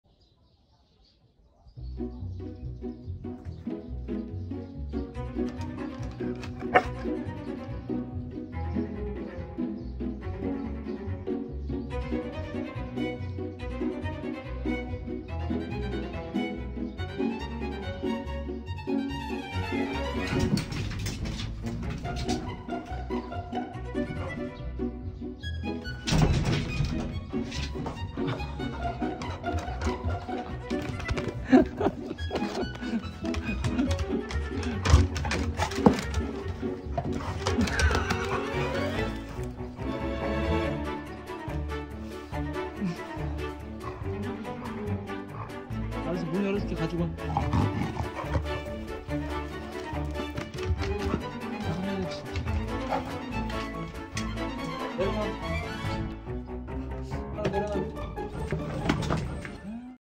문 안 열어주니까 신발로 딜 하는 리트리버 진짜 똑똑
하루의 지능을 확인 할 수 있는
짧은 영상
그 와중에 호야 왜케 귀엽니 ㅋㅋ